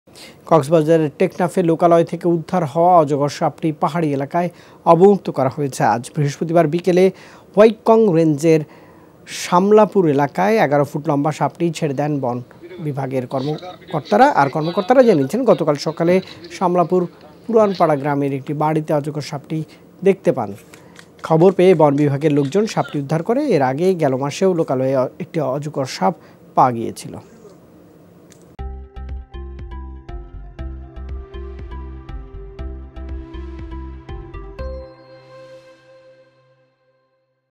পাহাড়ে অবমুক্ত হলো লোকালয়ের অজগর | CTG Python
কক্সবাজারের টেকনাফে লোকালয় থেকে উদ্ধার হওয়া অজগর সাপটি পাহাড়ী এলাকায় অবমুক্ত করা হয়েছে।